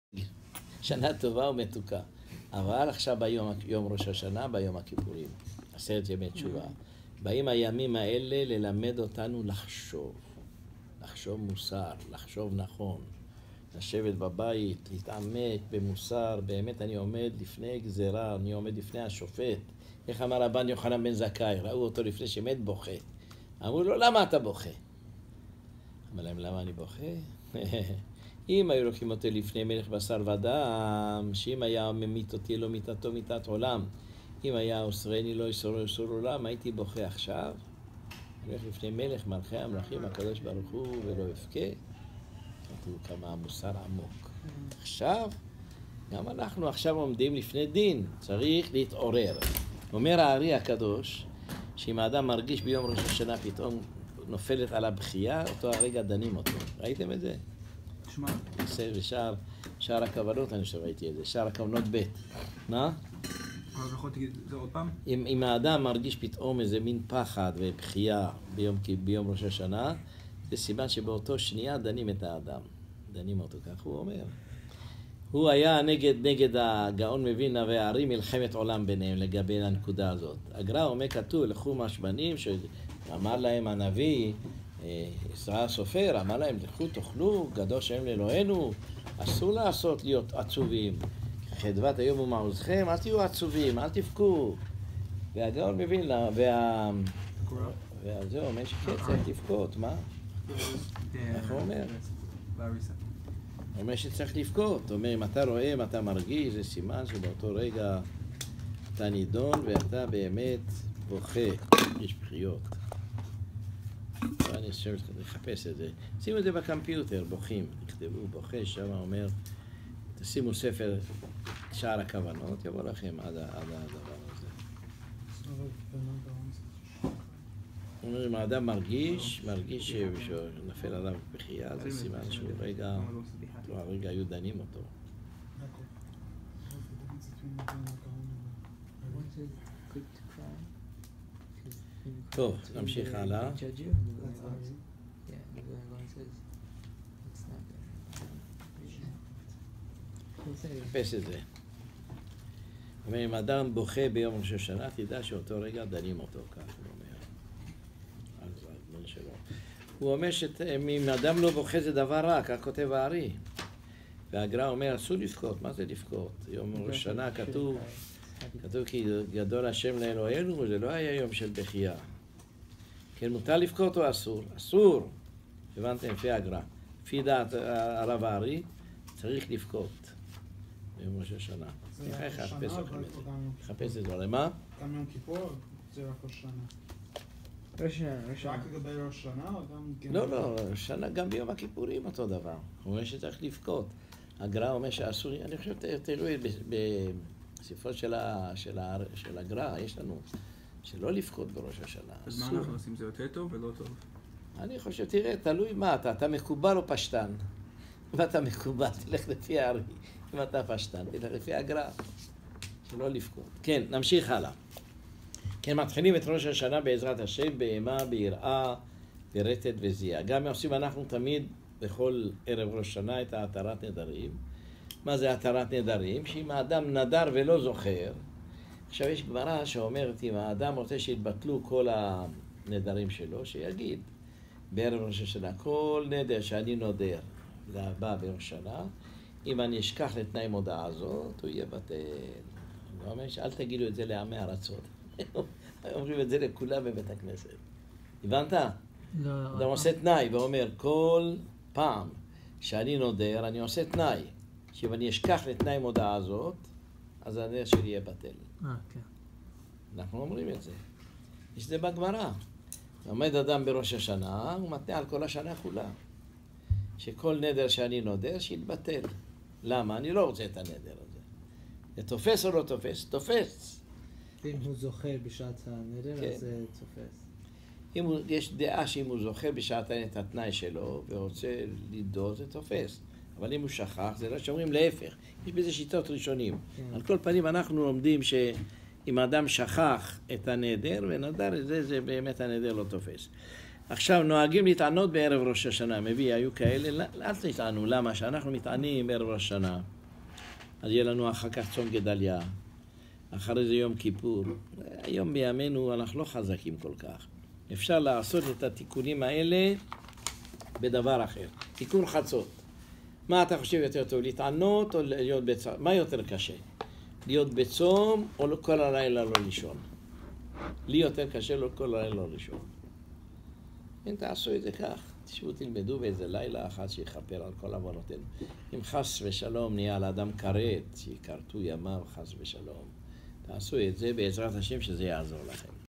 מוסר לראש השנה | הרב אליהו בן חיים
מוסר מהרב אליהו בן חיים לקראת ראש השנה.